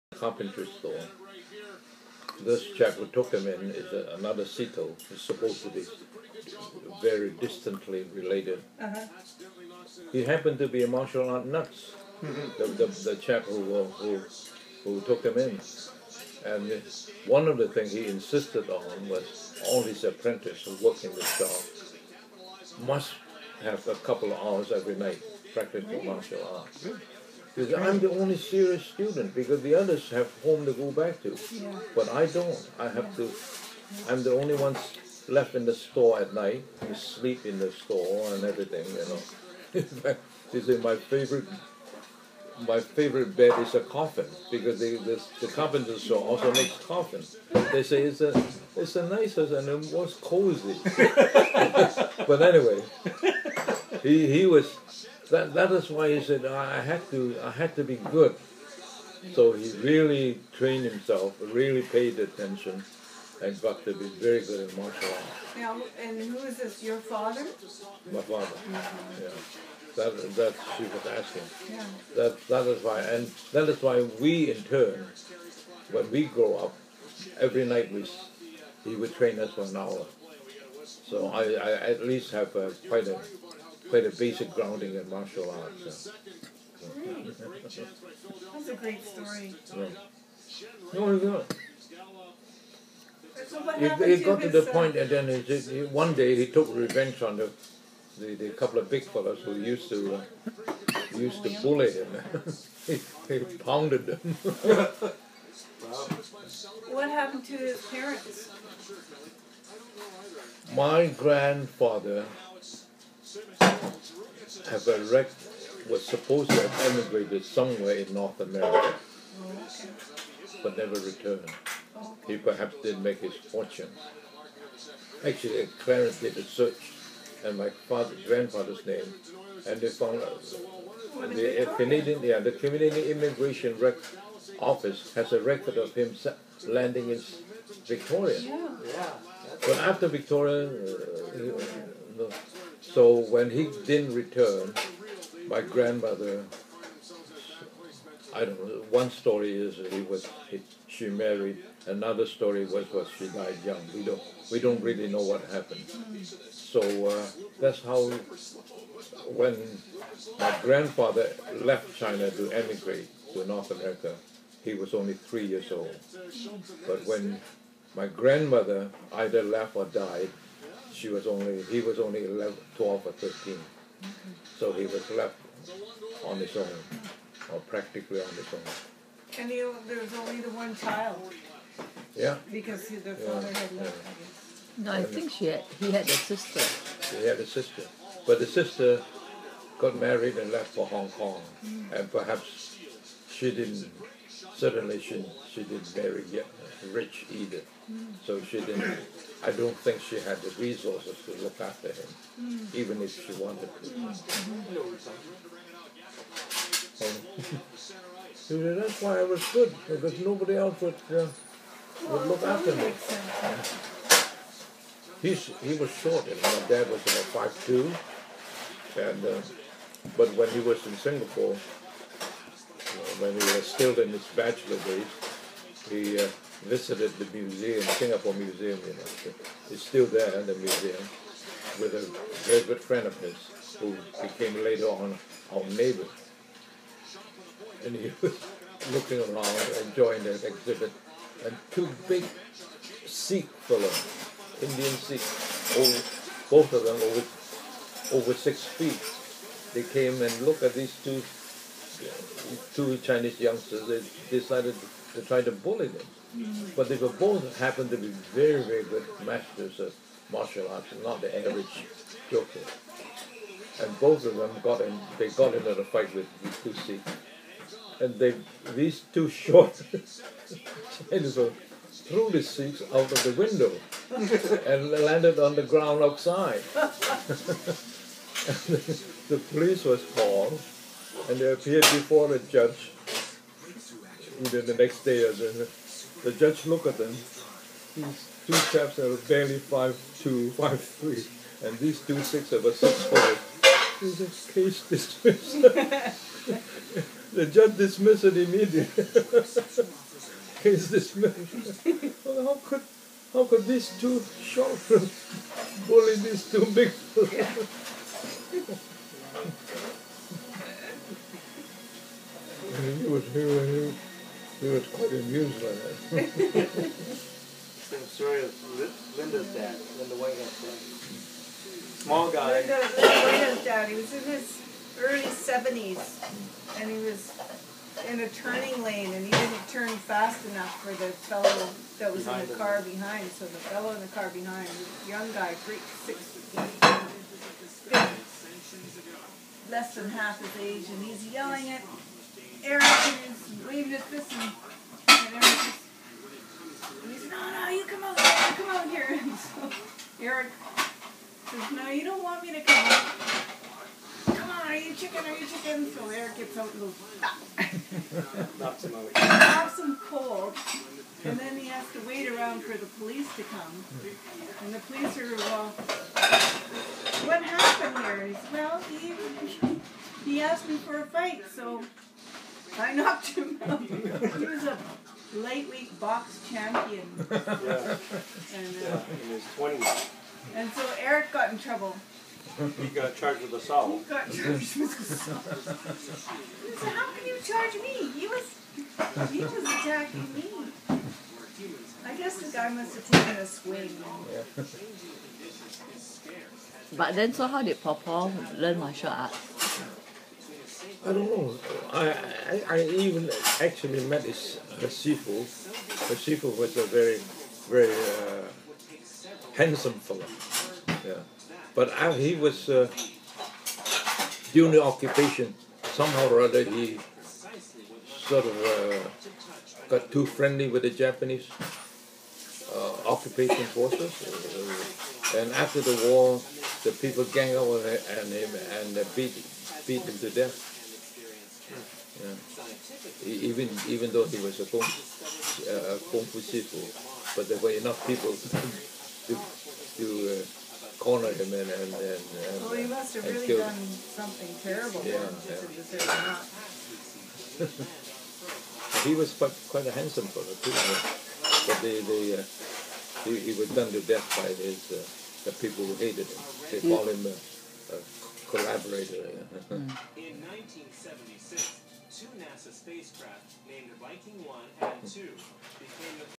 Uncle Colin on Grandpa's youth as apprentice carpenter
Dec 2013 during my visit to Vancouver, Canada